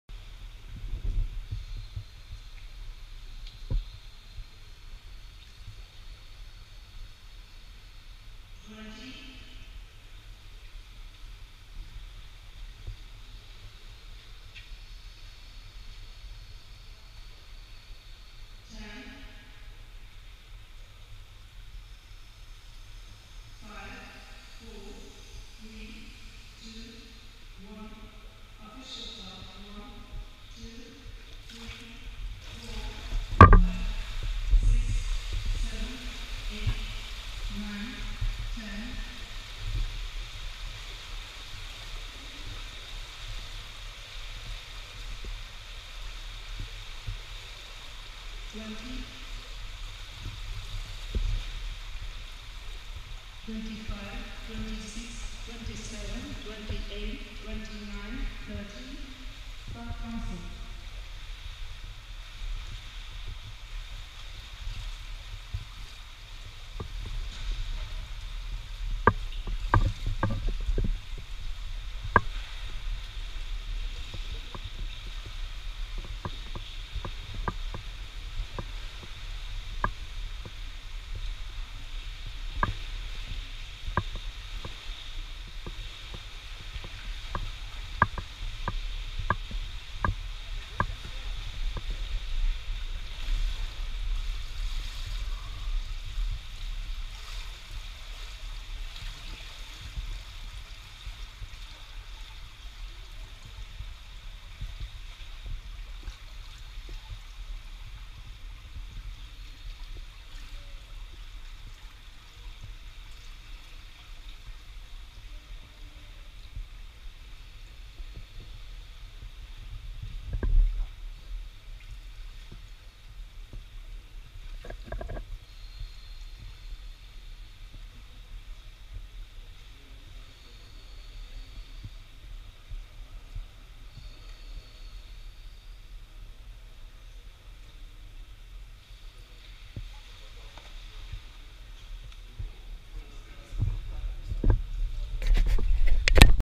Юрий Евтушенко. Динамика без ласт. Кубок Киева по фридайвингу 2016
Кубок Киева по фридайвингу 2016
Дисциплина: динамика без ласт (DNF)
Евтушенко Юрий
46 метров, белая карточка.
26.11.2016